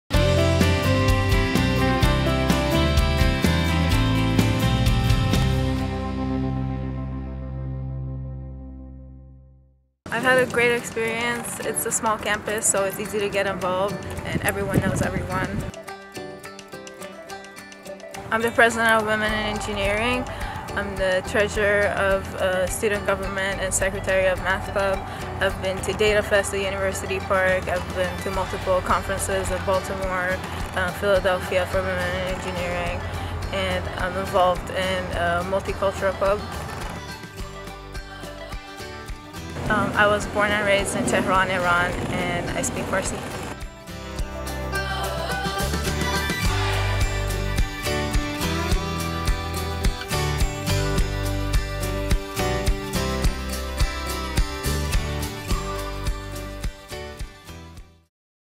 Brandywinner of the Week: Helia Aghababazadeh
This week's Brandywinner is Helia Aghababazadeh! An engineering major, Helia serves as president of Brandywine's Women in Engineering Club, treasurer of the Student Government Association and secretary of the Math Club. She's also an active member of the campus' Multicultural Club.